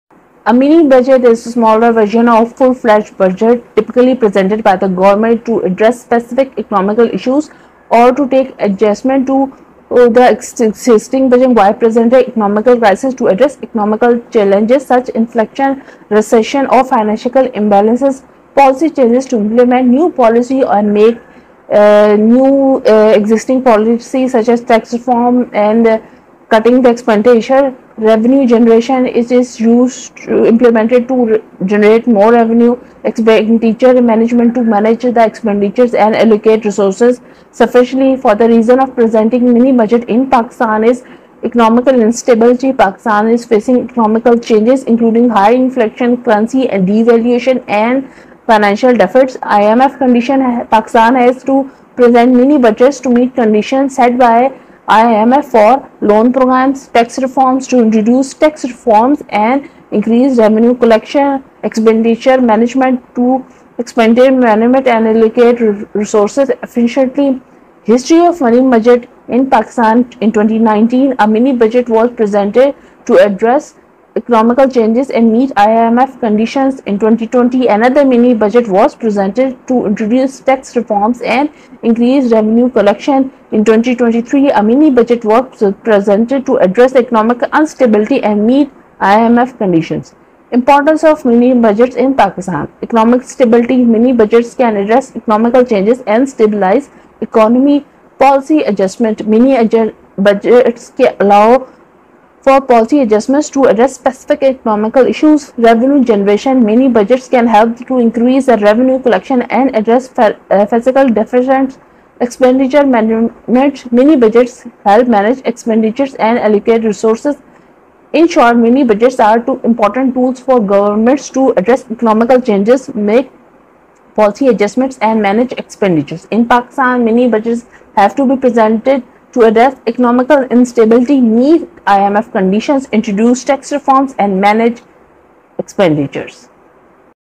Pakistan's Mini Budget: Relief or More Burden
This video can explore the key points of the mini budget, including:
- Taxation changes
- Spending allocations
- Economic growth initiatives
- Impact on various industries and citizens
- Comparison with previous budgets
- Expert analysis and opinions